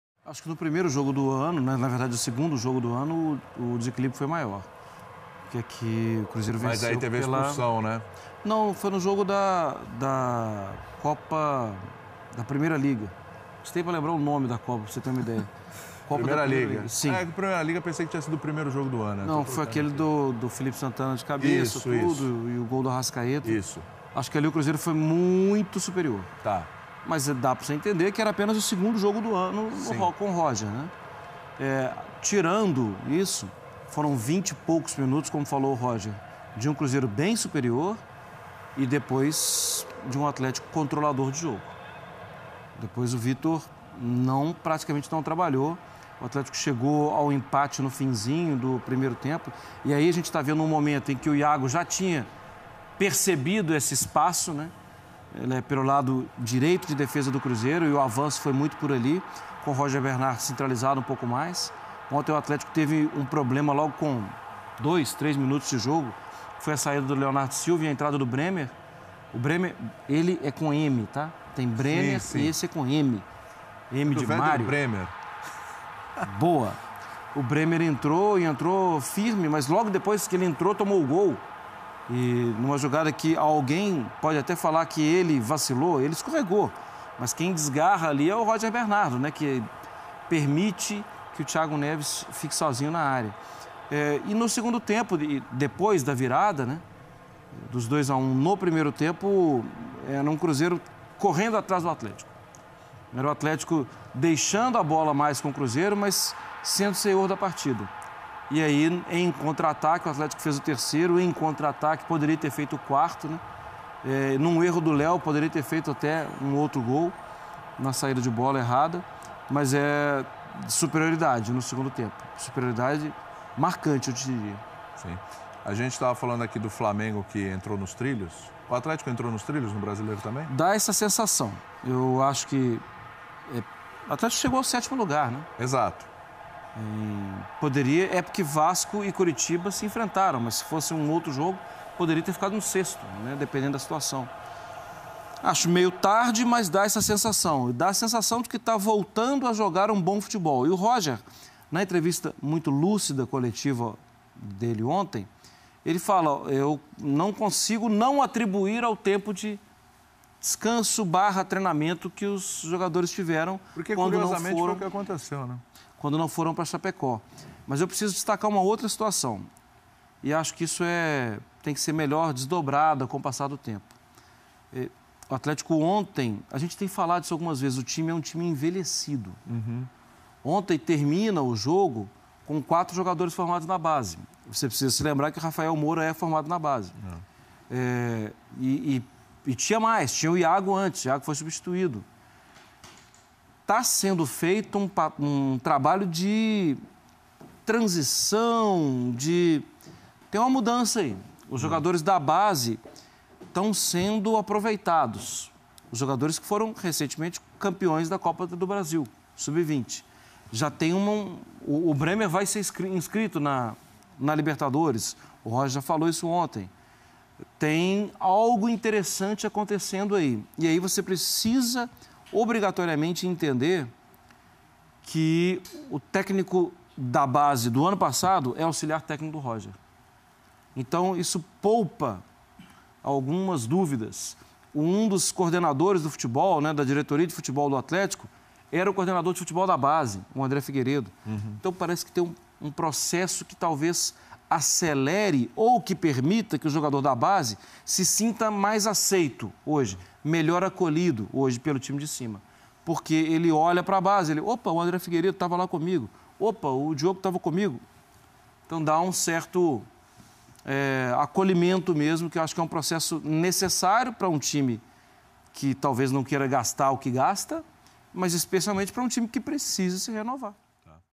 Na Marra! Virada do Atlético-MG para cima do Cruzeiro (Galo 3x1 Cru) BR 2017
Na Marra! Virada do Atlético-MG para cima do Cruzeiro (Galo 3x1 Cruzeiro) Brasileirão 2017